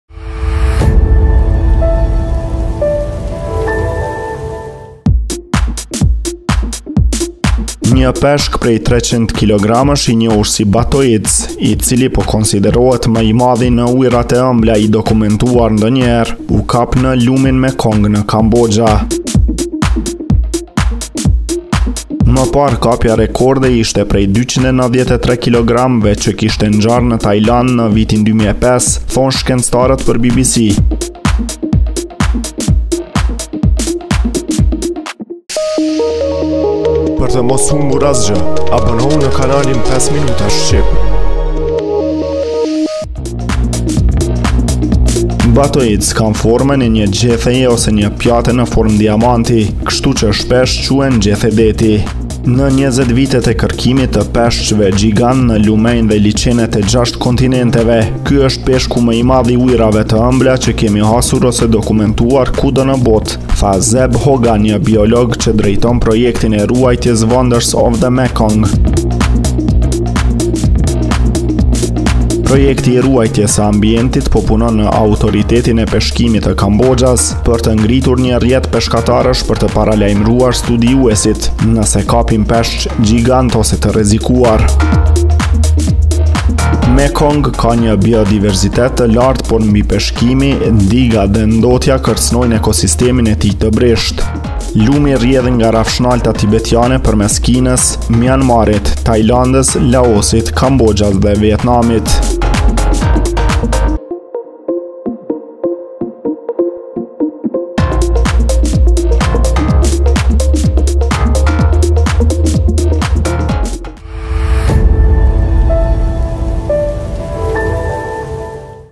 Gjendet peshku më i madh në botë i ujërave të ëmbla, ja si duket
Një peshk prej 300 kilogramësh i njohur si Batoids, i cili po konsiderohet më i madh i ujërave të ëmbla i dokumentuar ndonjëherë, u kap në lumin Mekong në Kamboxhia.

Më parë, kapja rekorde ishte prej 293 kilogramëve që kishte ngjarë në Tajlandë në vitin 2005, thonë shkencëtarët për BBC. Batoids kanë formën e një gjetheje ose një pjate në formë diamanti, kështu që shpesh quhen gjethe deti.

“Në 20 vitet e kërkimit të peshqve gjigantë në lumenj dhe liqenet e gjashtë kontinenteve, ky është peshku më i madh i ujërave të ëmbla që kemi hasur ose dokumentuar kudo në botë”, tha Zeb Hogan, një biolog që drejton projektin e ruajtjes ëonders of the Mekong.

Projekti i ruajtjes së ambientit po punon me Autoritetin e Peshkimit të Kamboxhias për të ngritur një rrjet peshkatarësh për të paralajmëruar studiuesit nëse kapin peshq gjigantë ose të rrezikuar.

“Gjetja dhe dokumentimi i këtij peshku është një shenjë e jashtëzakonshme dhe e rrallë shprese, aq më tepër sepse ndodhi në Mekong, një lumë që aktualisht po përballet me sfida të shumta”, shtoi Dr. Hogan, i cili është gjithashtu profesor në Universitetin e Nevadës.

Mekong ka një biodiversitet të lartë, por mbipeshkimi, digat dhe ndotja kërcënojnë ekosistemin e tij të brishtë. Lumi rrjedh nga Rrafshnalta Tibetiane përmes Kinës, Mianmarit, Tajlandës, Laosit, Kamboxhias dhe Vietnamit.

Natën e 13 qershorit, një peshkatar lokal në ishullin Koh Preah thirri studiuesit dhe tha se kishte kapur thekër “shumë të madhe”.

Doli të ishte 3.98 metra e gjatë dhe 2.2 metra e gjerë.

Pasi u pajis me një etiketë gjurmuese, thekra u lëshua përsëri në lumë.

me ler te flas